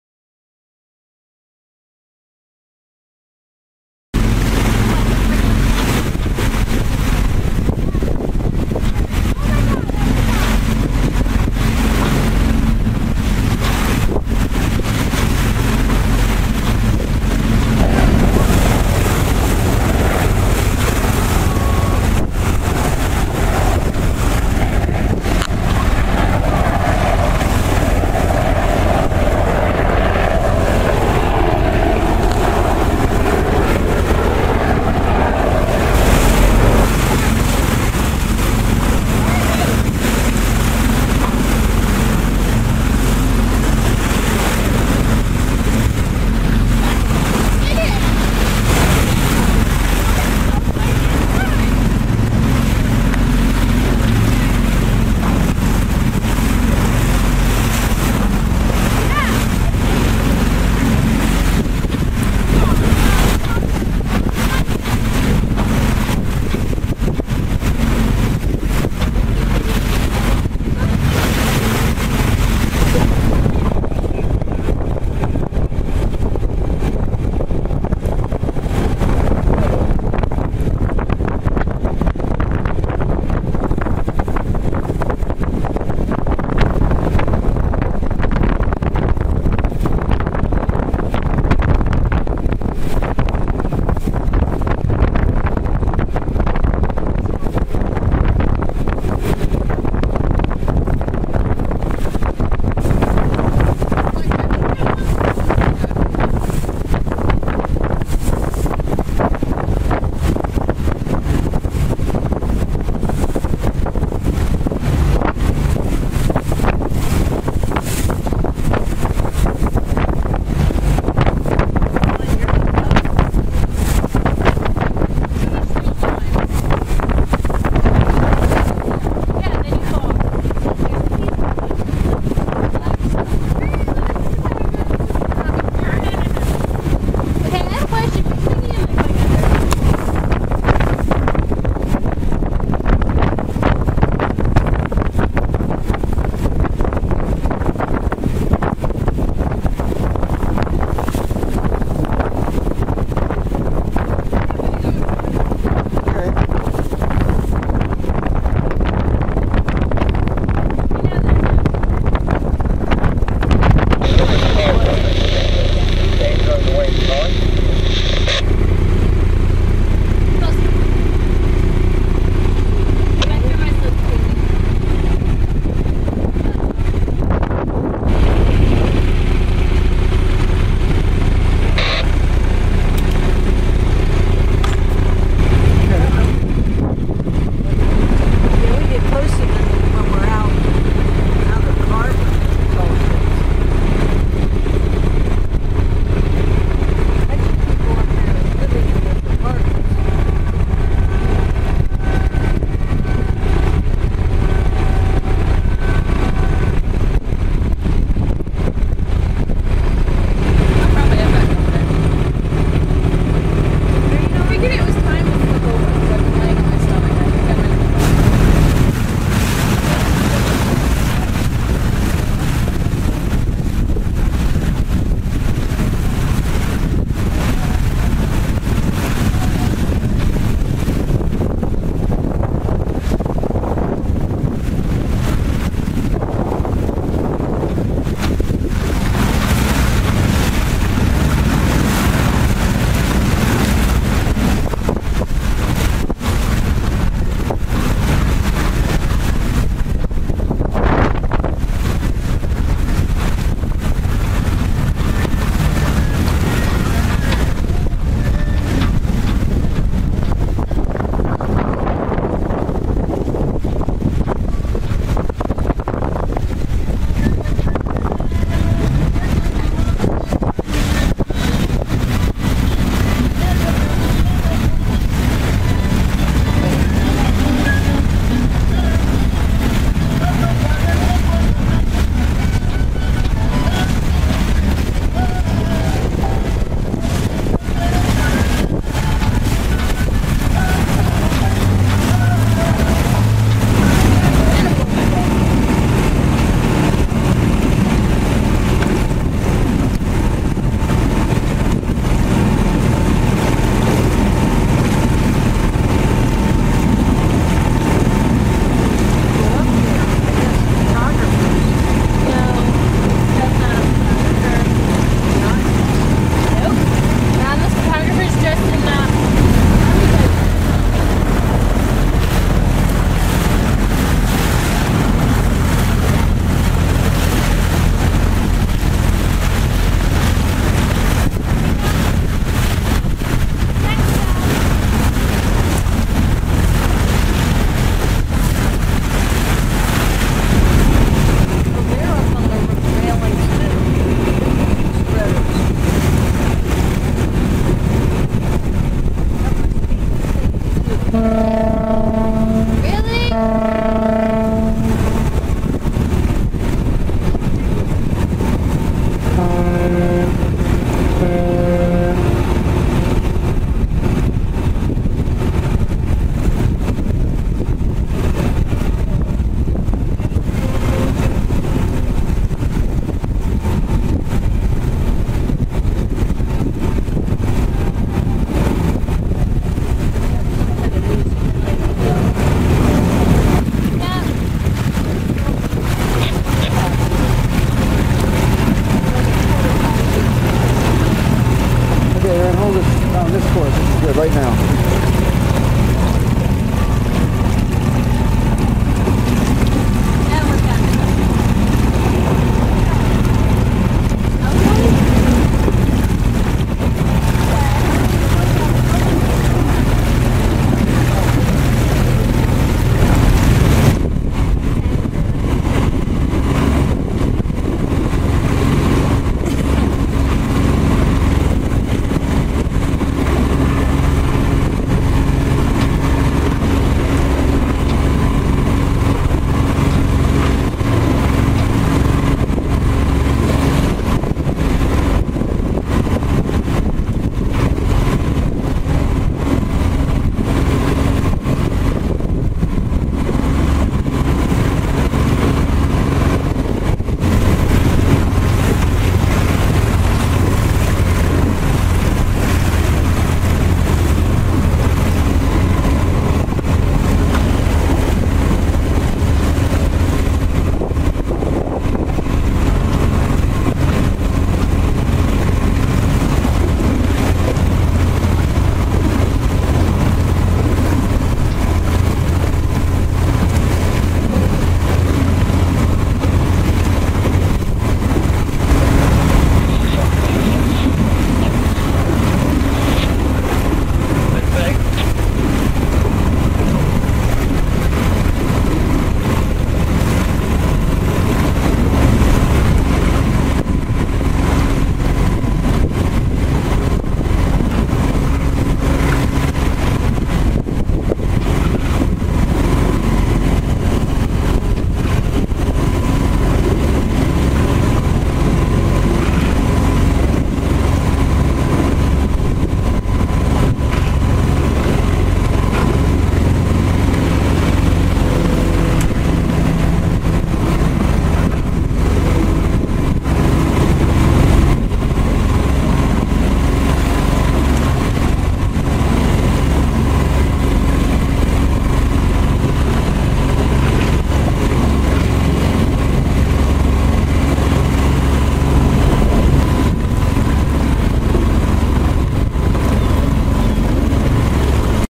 TallShips3.mp4
Tall Ships arrival into Baltimore Harbor June 13, 2012
Video taken from Beck 'N Call by Roy Wainwright
Erin Bankert at the helm